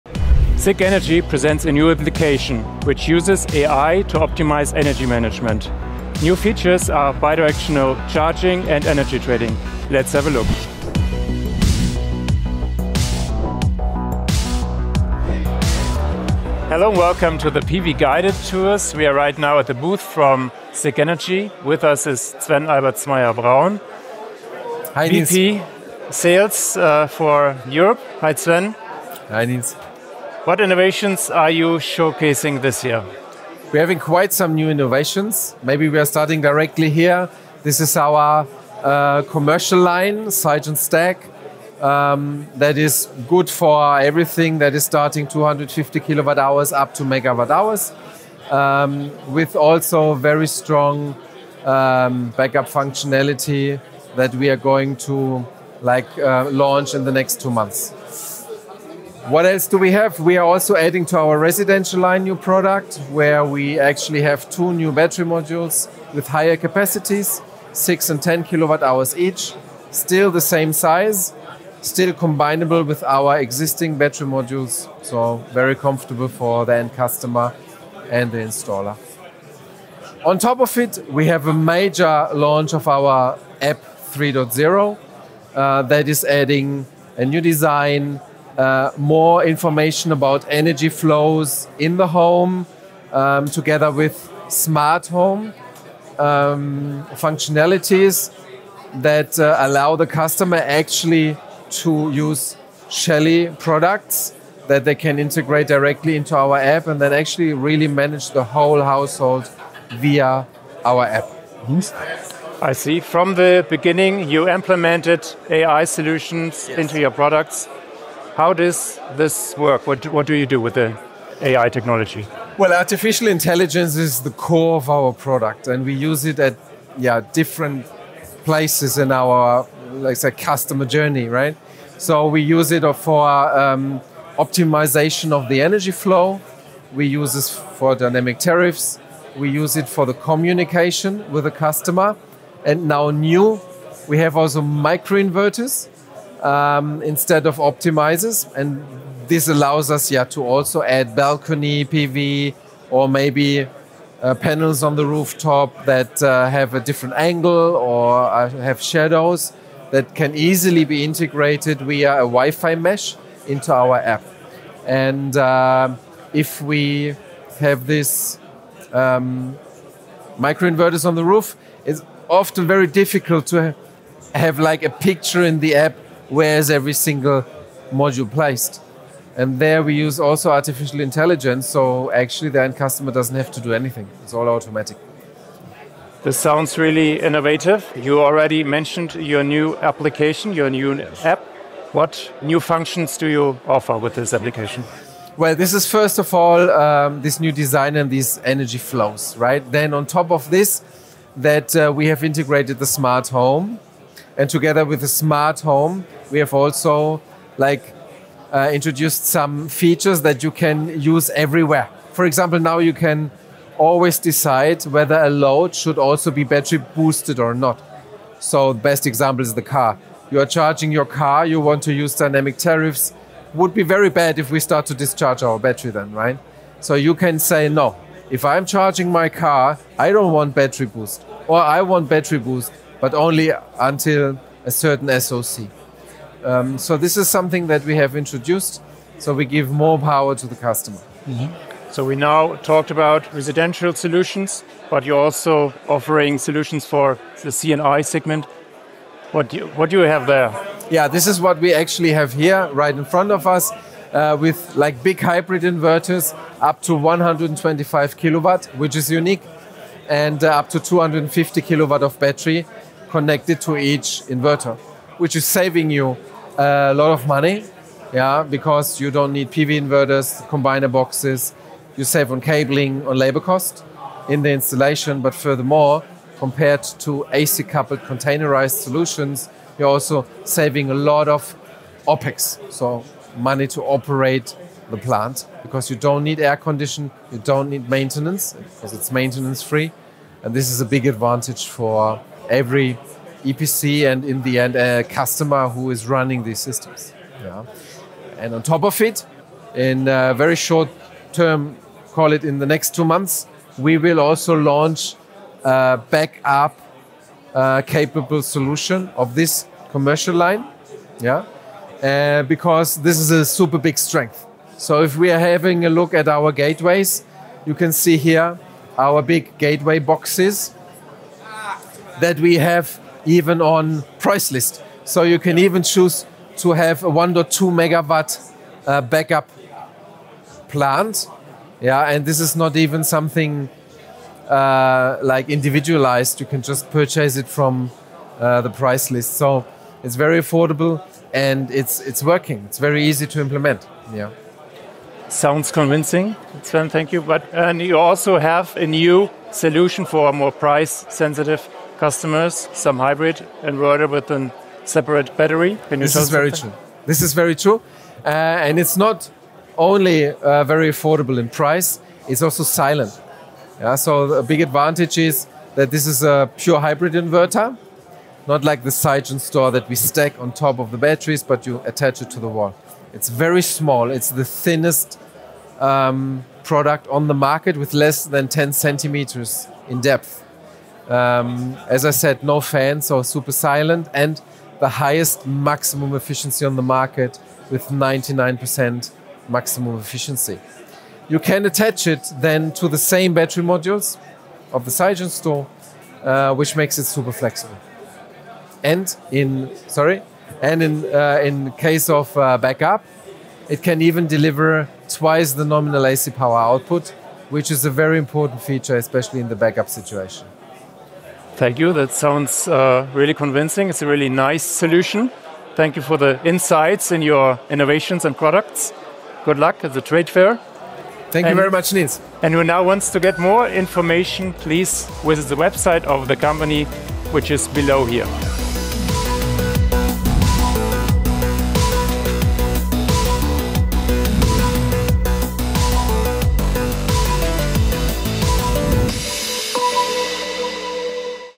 New app optimises energy management with AI | Sigenergy
Sigenergy considers itself well prepared for the new energy world. The new app uses AI to optimise energy management. New functions enable the integration of optimisers, bidirectional charging and energy trading. For price-conscious customers, the second-generation hybrid inverter with an extra battery is now available, which can also be easily retrofitted.